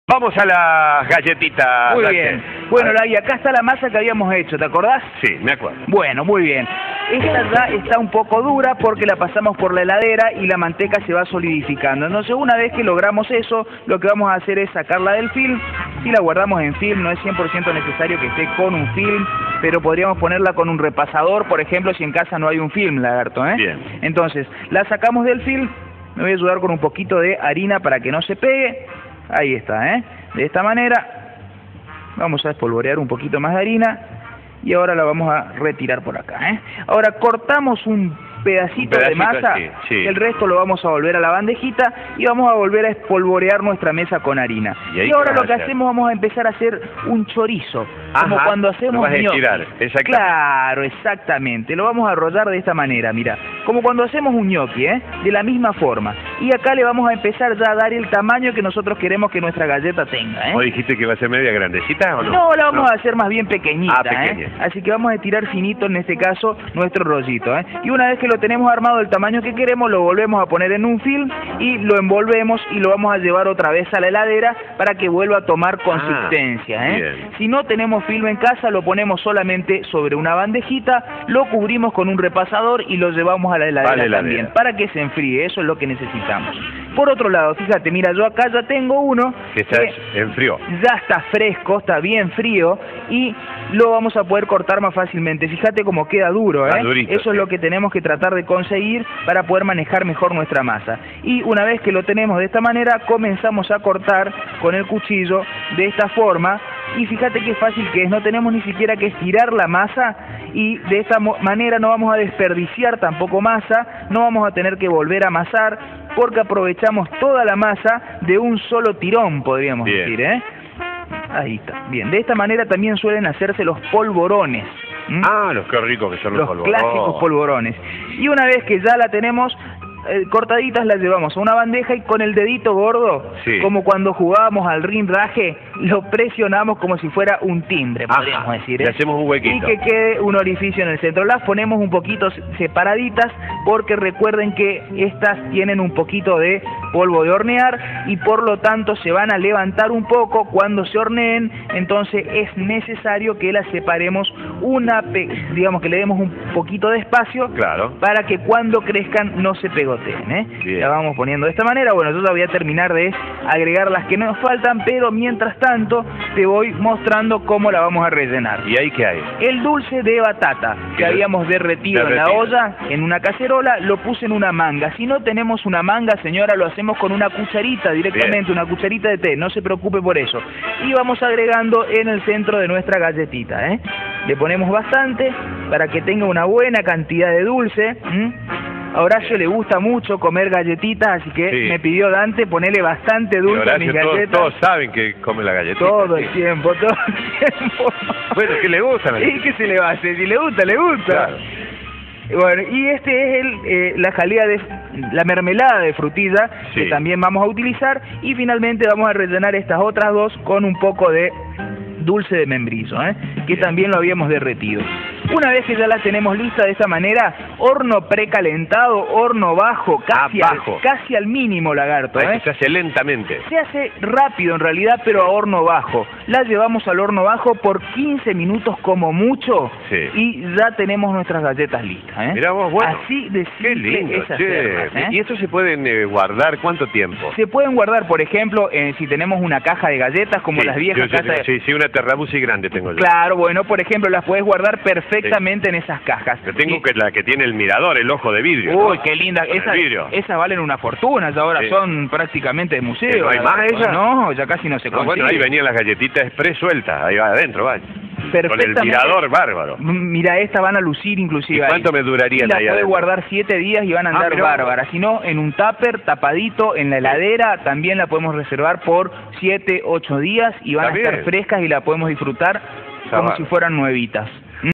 pepitas de maiz pt3 20110429.3gp
Hoy cocinamos una receta ideal para acompañar el desayuno o la merienda de los días de frío: "Pepitas de maíz". La receta rinde para cuatro personas.